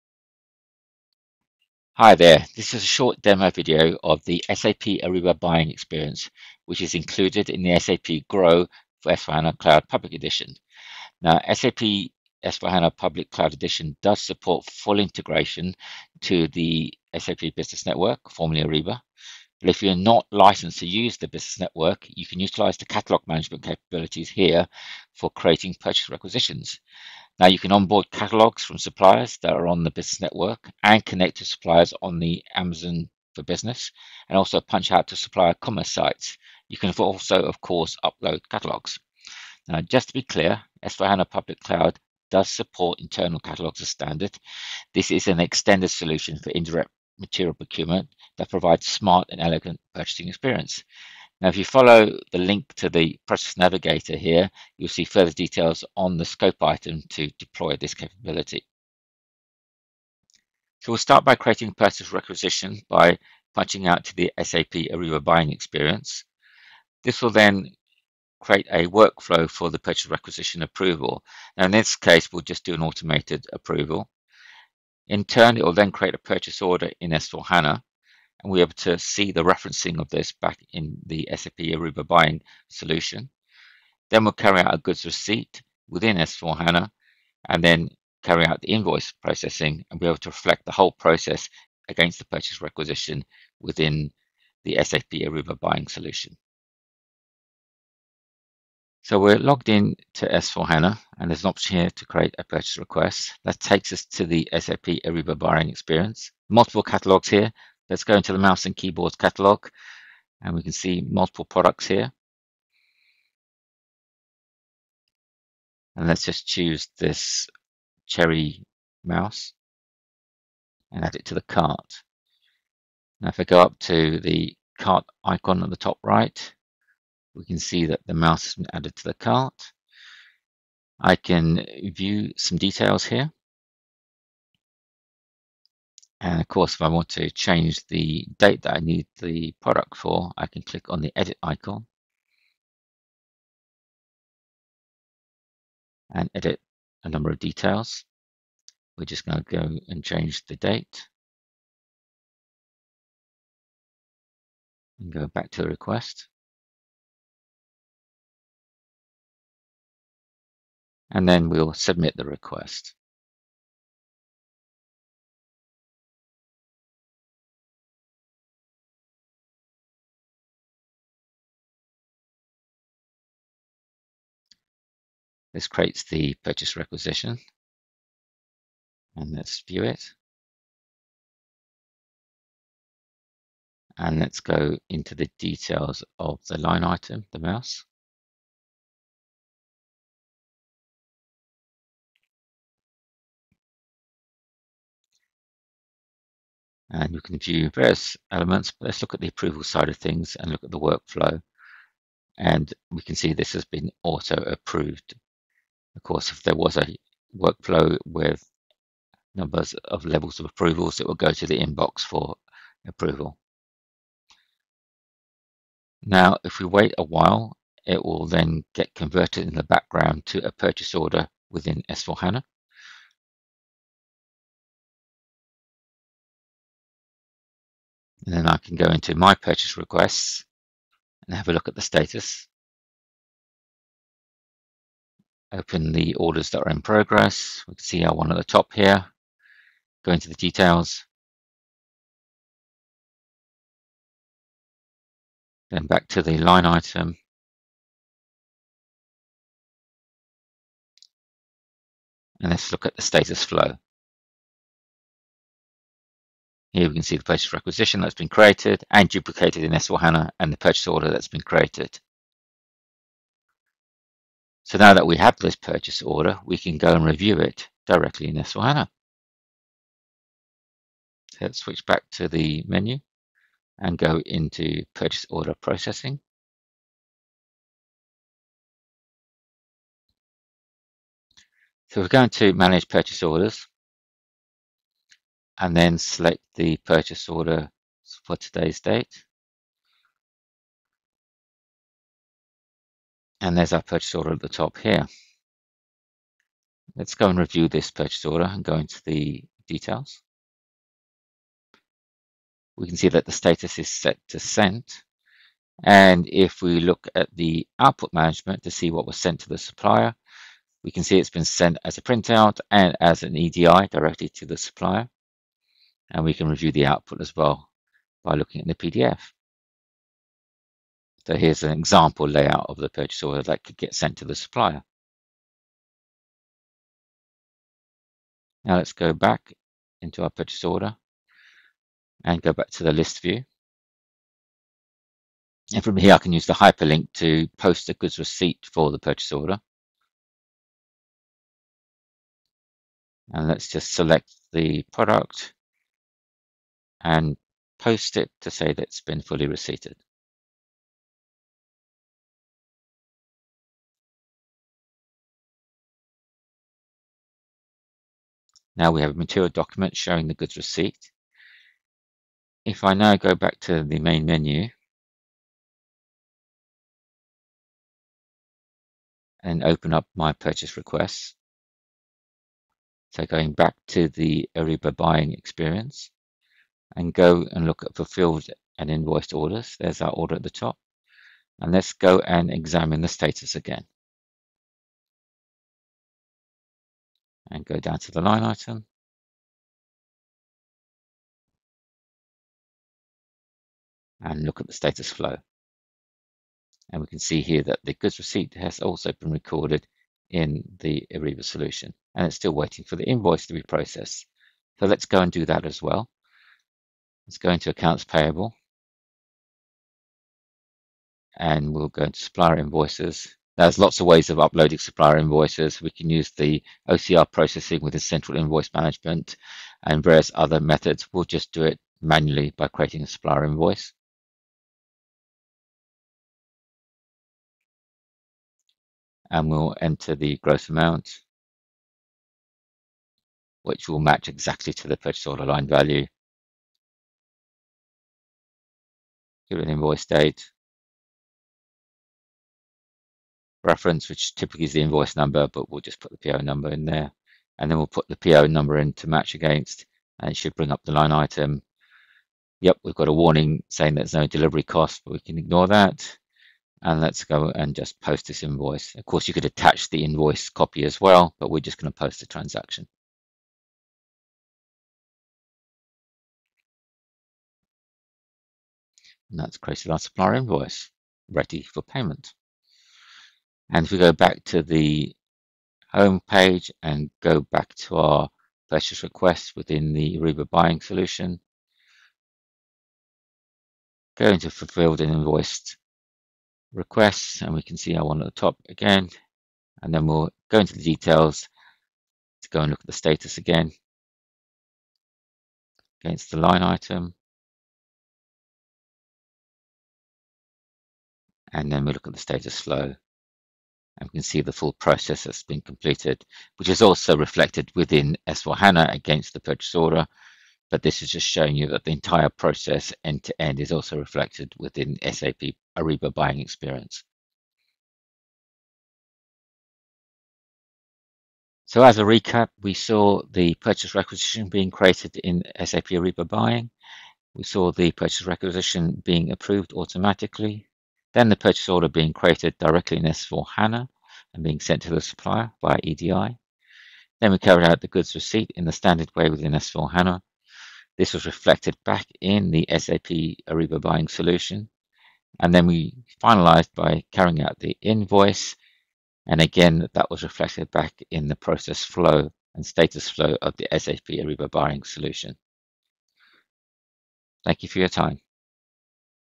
S/4HANA Public Cloud with Ariba Buying Based Edition
This is a short demo video of the ‘SAP Ariba Buying’ experience which is included in SAP Grow S/4HANA Cloud Public Edition. SAP Grow S/4HANA Cloud Public Edition does support full integration to the SAP Business Network (formerly Ariba),  but if you are not licensed to use the business network you can utilize the catalog management capabilities, for creating purchase requisitions.   You can on-board catalogues from suppliers that are on the business network, and connect to suppliers on Amazon Business and punch out to supplier commerce sites.  You can also upload catalogues.

Just to be clear, S/4HANA Public Cloud does support internal catalogues as standard.  This is an extended solution for indirect material procurement that provides  a smart and elegant purchasing experience.